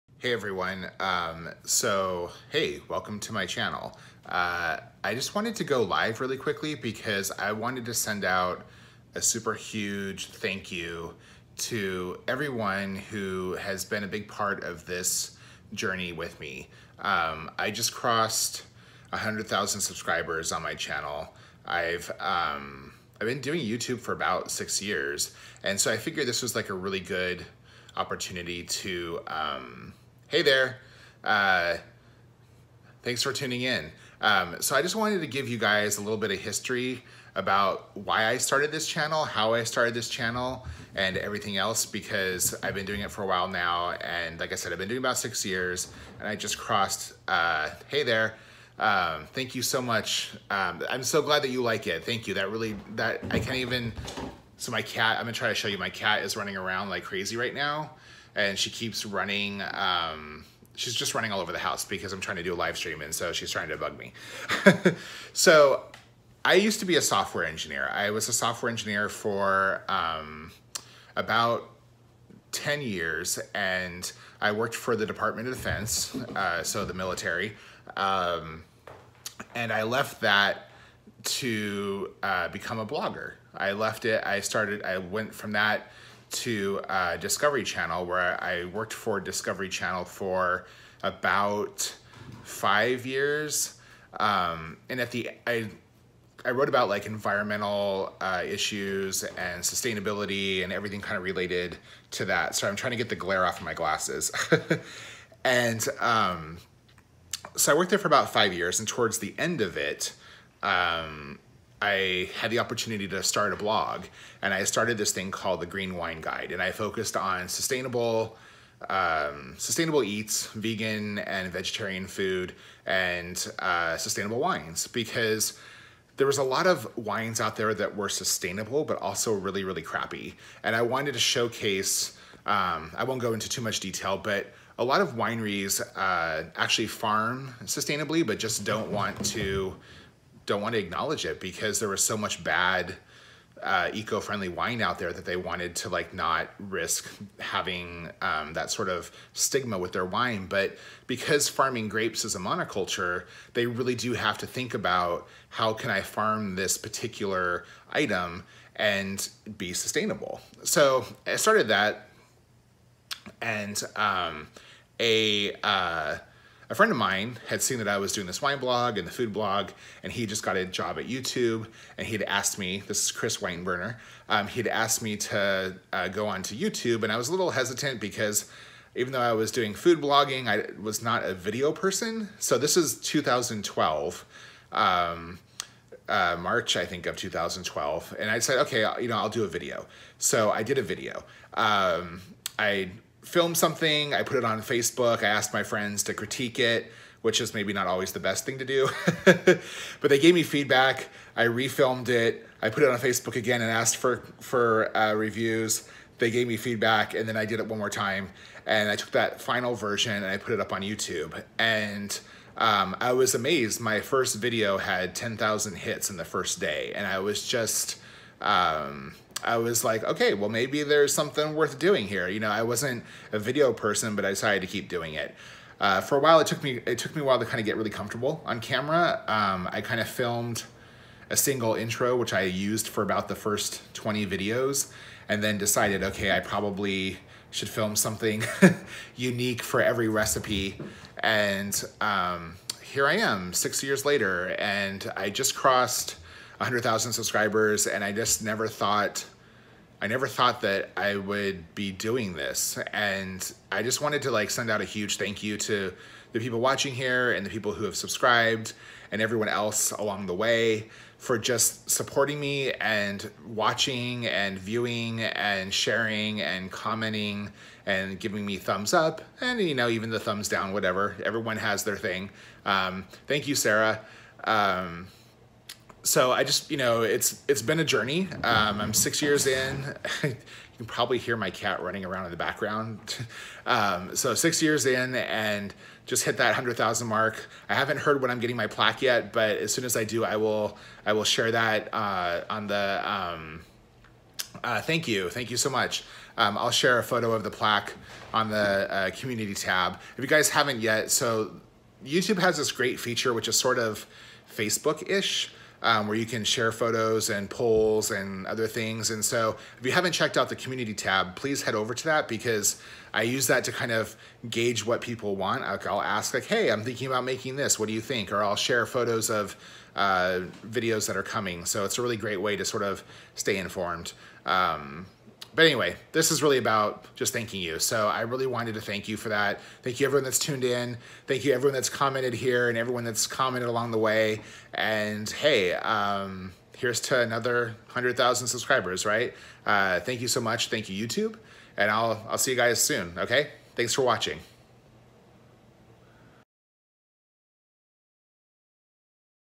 100,000 Subscribers 🎉
Oh my God you guys, I just crossed hundred000 subscribers and I can’t even believe it. I just want to say thank you!